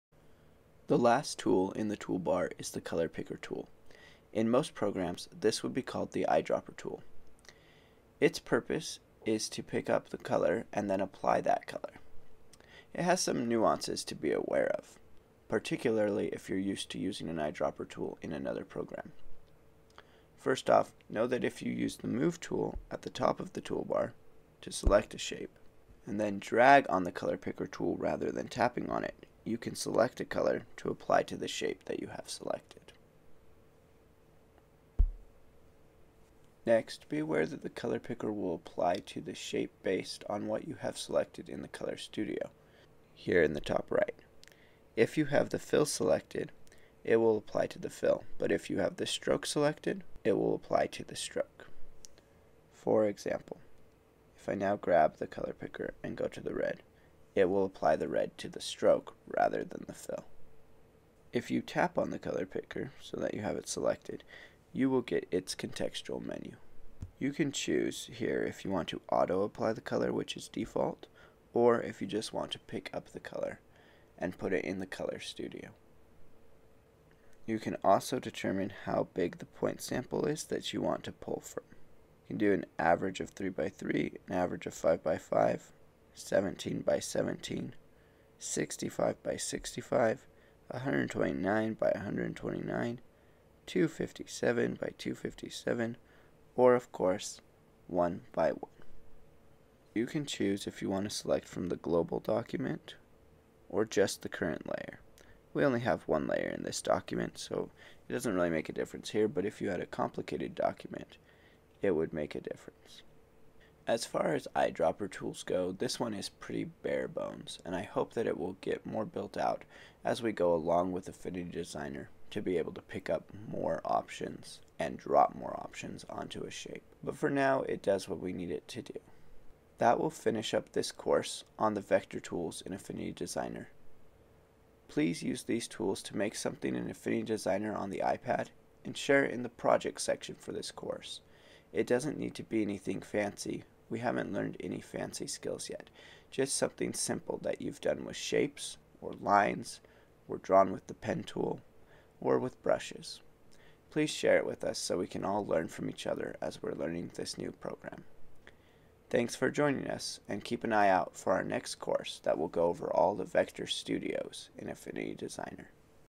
Color Picker (Eye Dropper) Tool in Affinity Designer for iPad
The color picker tool in Affinity Designer for the iPad allows you to lift colors from one area of your design and apply them to other areas of your design, allowing you to get the perfect match.